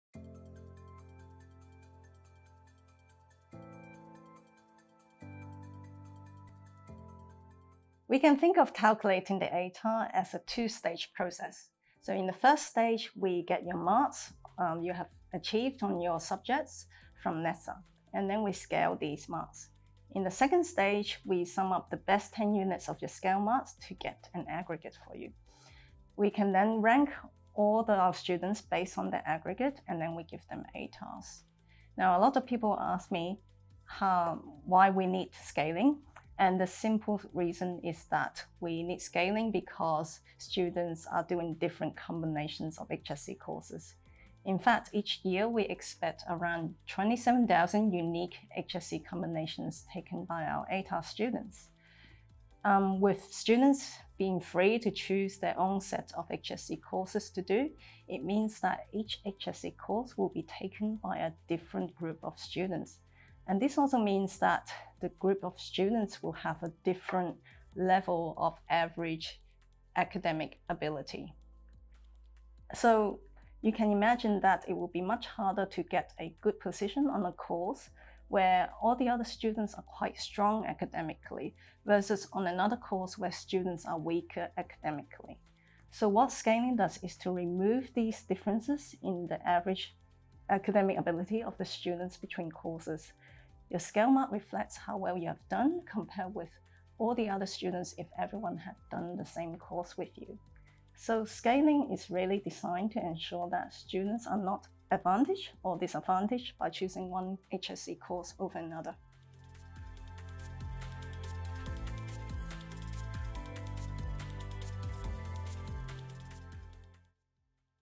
How does UAC calculate the ATAR and why does it scale HSC marks?
HSC students hear a lot of confusing messages about the ATAR and scaling. UAC’s ATAR expert – Dr Helen Tam – is here to give you the facts.